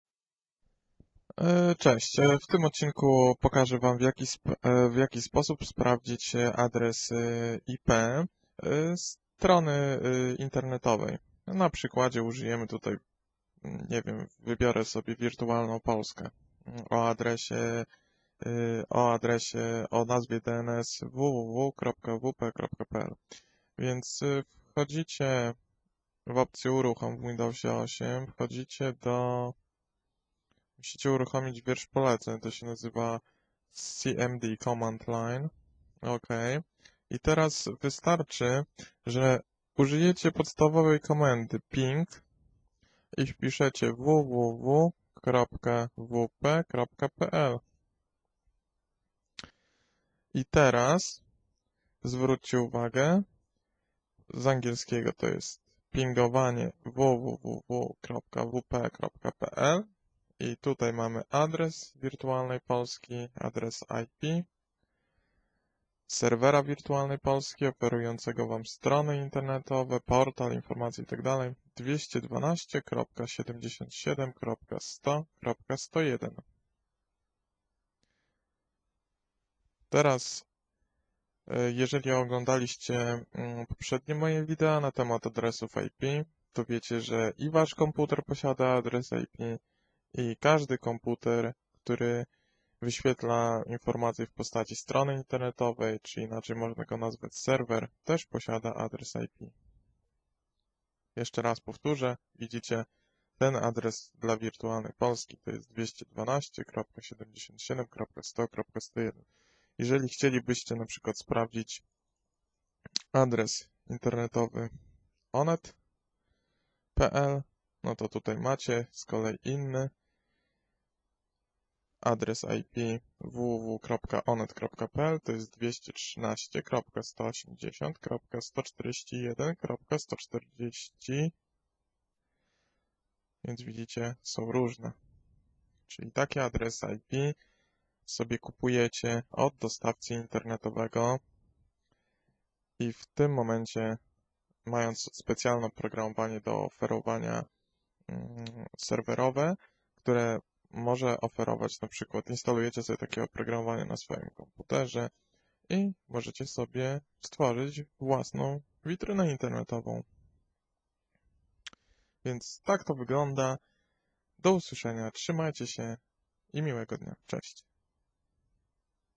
ADRES IP STRONY WWW ? TO PROSTE
Pokazuję w jaki sposób możecie się dowiedzieć jaki jest adres IP np. strony internetowej.
Pozdrawiam.